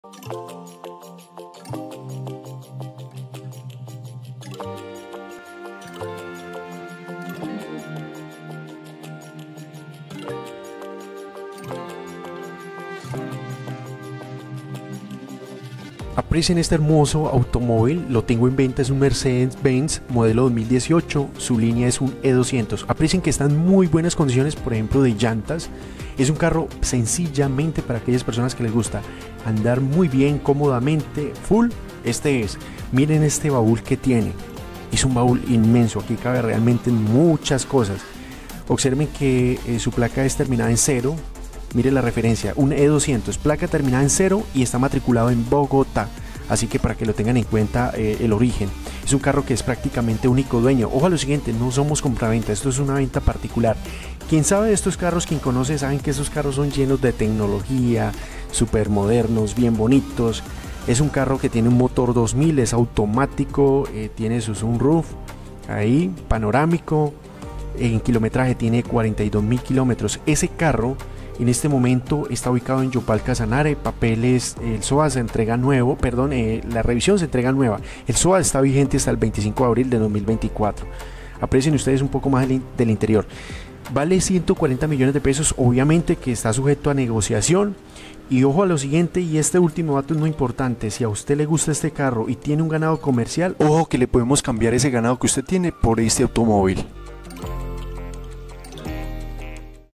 🔥(SE VENDE)🔥ESTÉ BONITO AUTO🔥INFO: llamar directamente al: ( 🔥
SE VENDE - SE CAMBIA POR GANADERÍA COMERCIAL. Es modelo (2018), con todos sus documentos al día, prácticamente único dueño. Soat vigente hasta el: 25-ABRIL-2024 - Revisión vigente hasta el: se entrega nueva.

Es un carro muy full, con bastante tecnología, los conocedores de estos autos saben de qué estamos hablando. De pintura, de mecánica, llantas y demás, está en muy buenas condiciones, mínimos detalles. Está matriculado en “Bogotá”, terminado en placa (PAR - 0). El precio es de: $140´000.000.